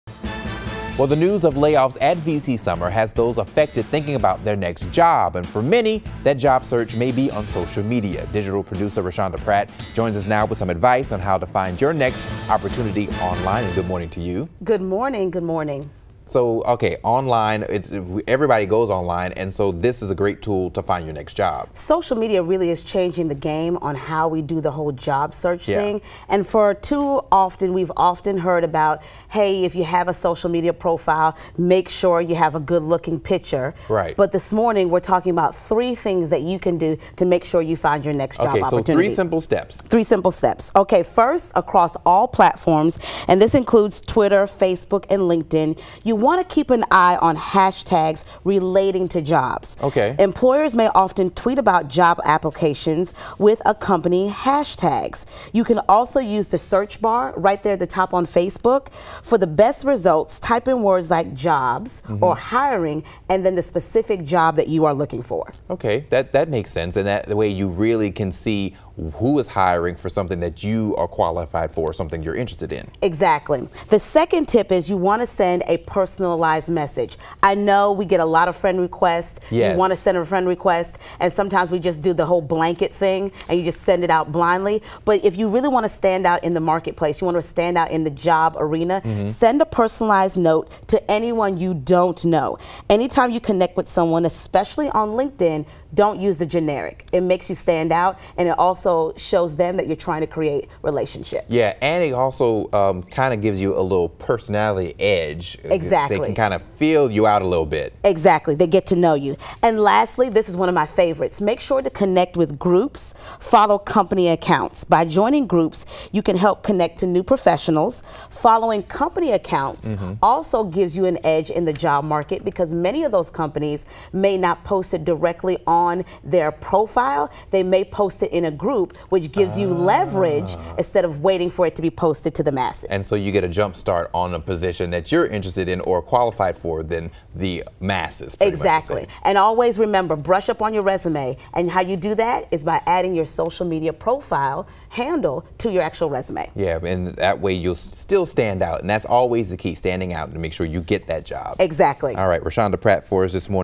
3 ways to Use Social Media to Hear You're Hired!
With massive layoffs from a local plant and the changing job market employees are looking for creative ways to stand out in the job market.  News 19 Digital Producer Roshanda Pratt shows 3 ways to use social media to hear you're hired.